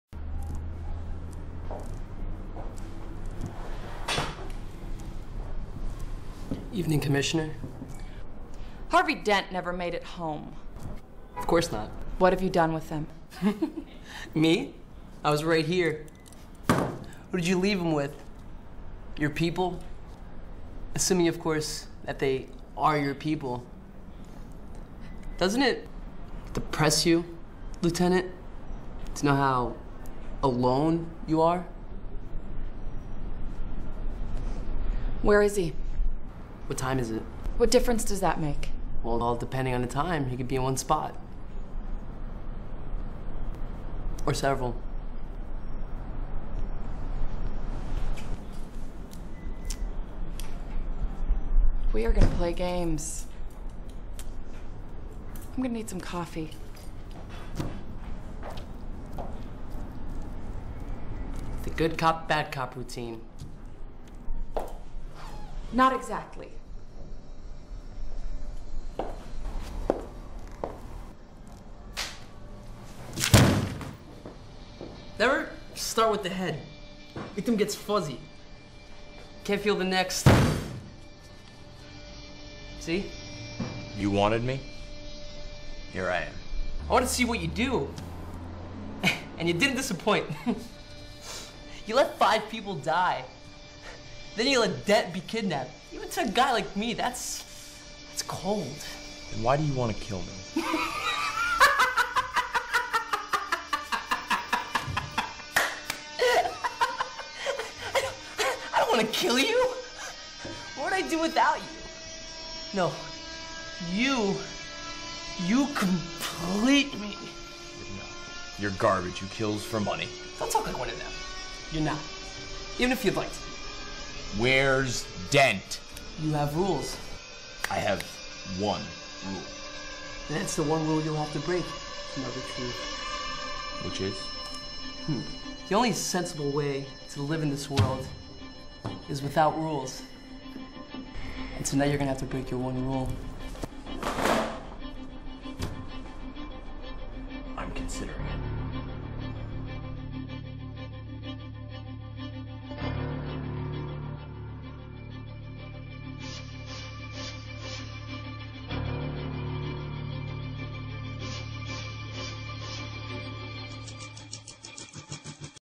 Production workshop- TDK (interrogation scene)
This is a recreation of the interrogation scene from the movie The Dark Knight. Made for my Filmmaking Class on 2010.

Directed by: Eddy Martin Molina
Starring: Jonelle Browne, Steve Serra, Dan McCallian